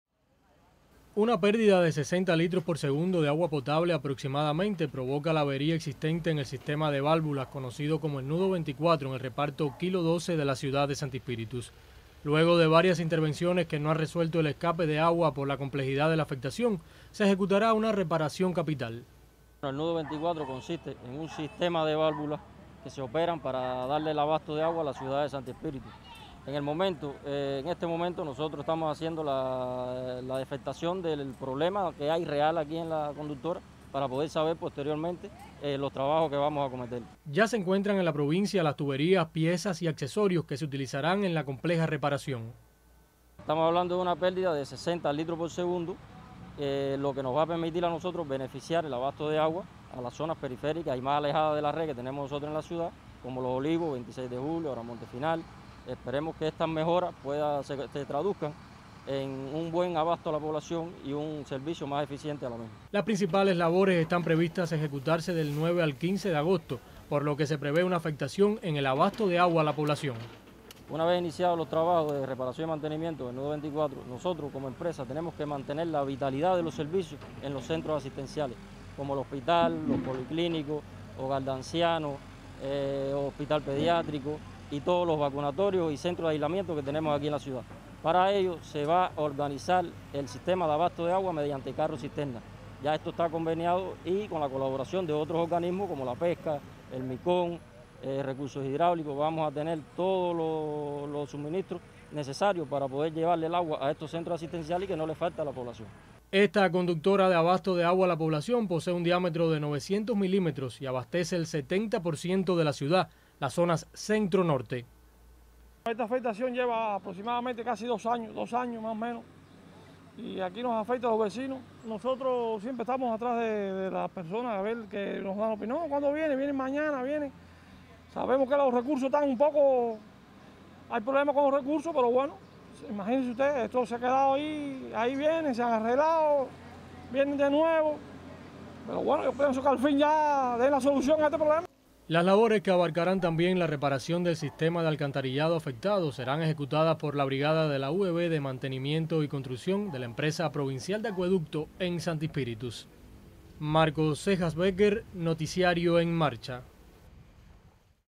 Comienzan labores de reparación y mantenimiento en el sistema de válvulas hidráulicas de la ciudad.
Ante la compleja avería que presenta el sistema de válvulas, conocido como el nudo 24 en el Reparto Kilo 12 de la ciudad de Sancti Spíritus, se decidió su reparación capital, prevista entre los días del 9 al 15 de agosto